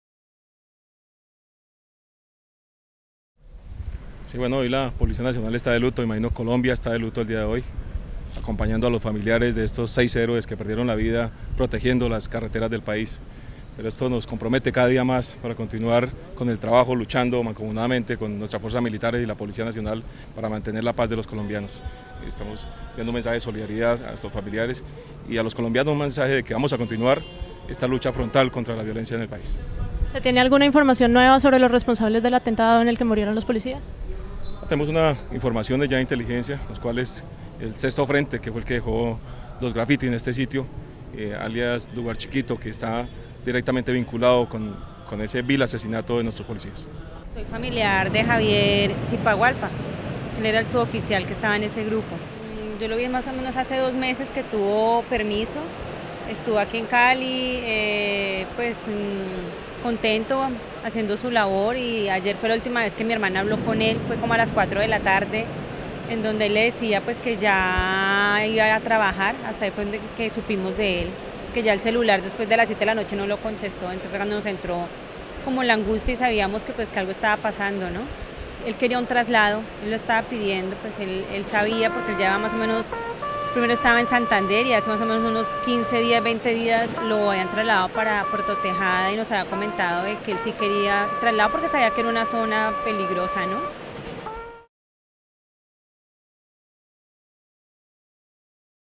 Despiden con honores a los seis policías asesinados en el norte del Cauca
Los familiares y compañeros de los seis policías que murieron en un ataque de las Farc, en la vía que conduce de Puerto Tejada a Padilla, despidieron en Cali con un acto litúrgico a sus seres queridos. Después de esto, los uniformados fueron trasladados a sus ciudades de origen.